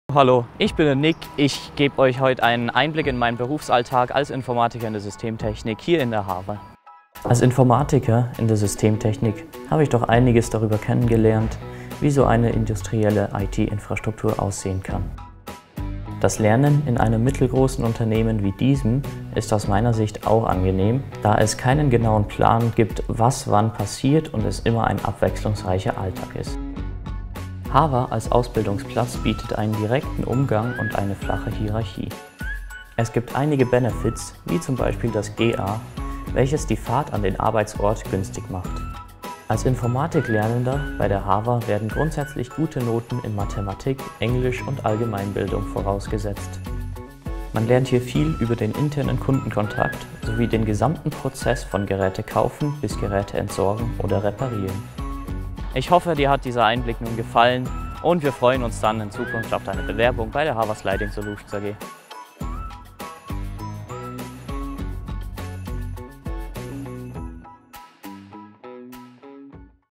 Computeraffin? Informatiker/in EFZ! | Lehrstellenvideo | Yousty
Ein Einblick in den Alltag einer Lehrstelle bei der Hawa Sliding Solutions in Mettmenstetten, Zürich als Informatiker/-in EFZ